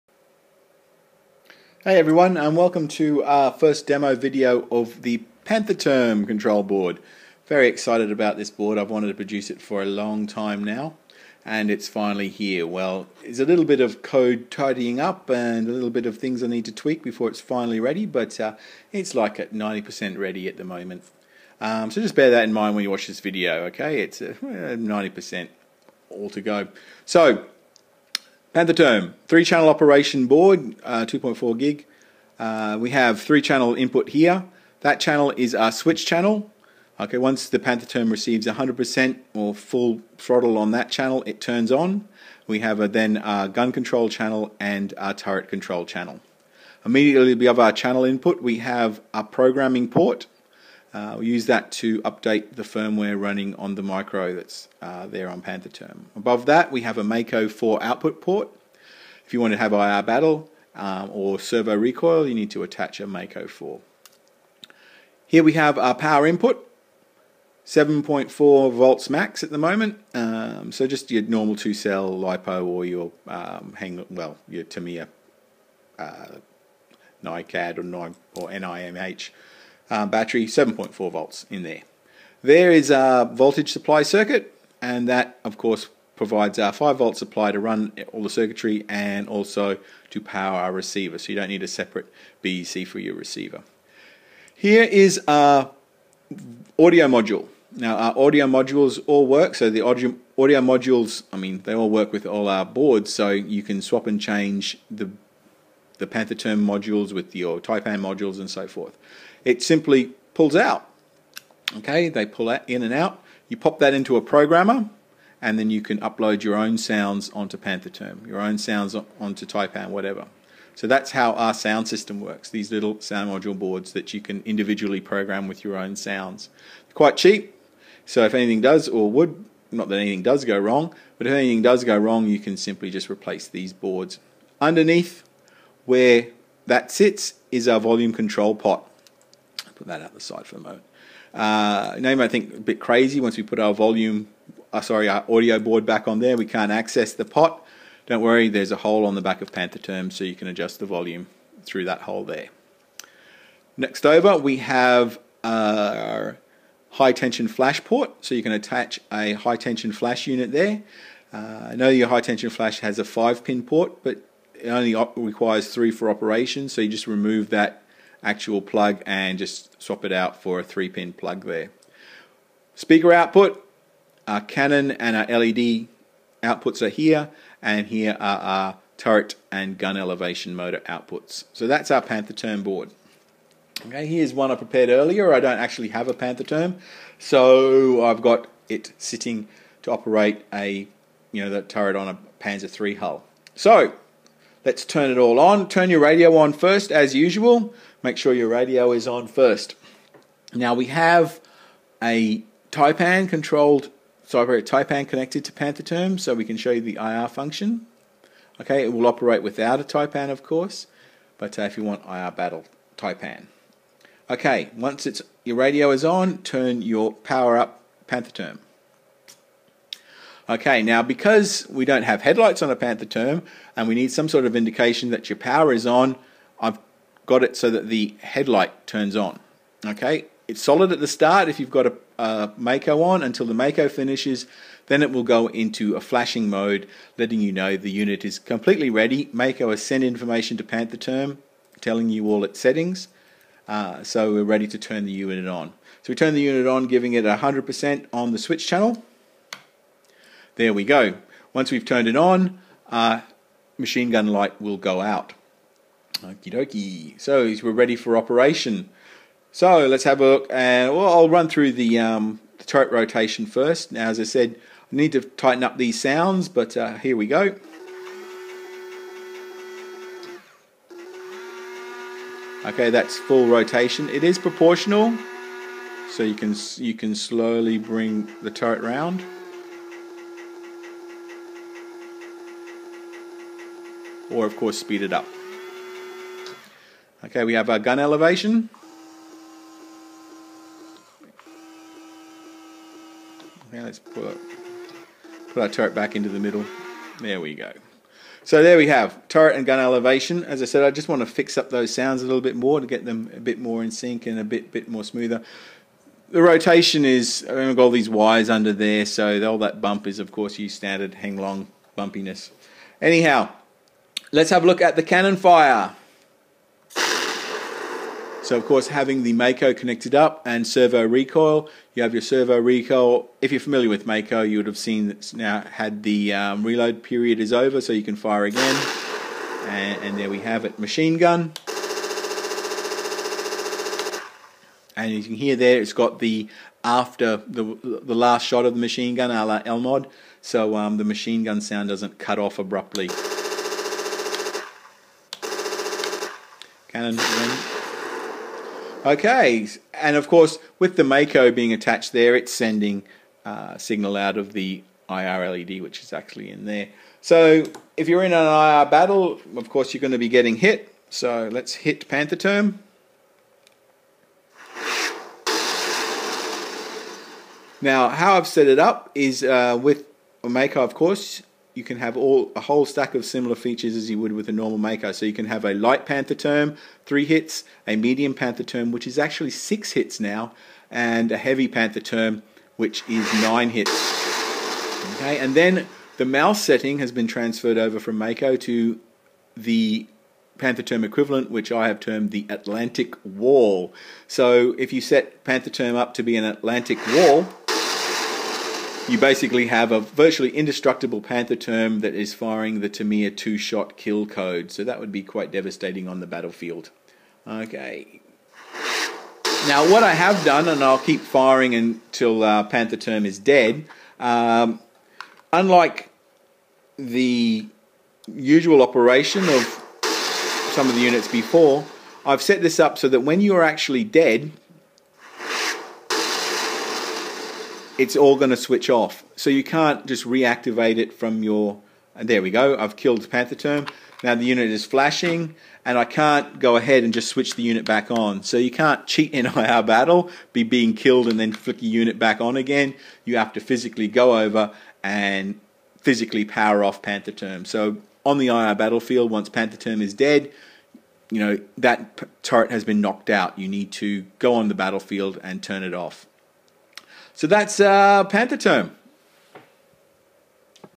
Pantherturm Introduction
An intro video of our new Pantherturm Control Board.  3 channel operation , 7.4 volt input, cannon LED and high tension flash support.  User changeable sounds.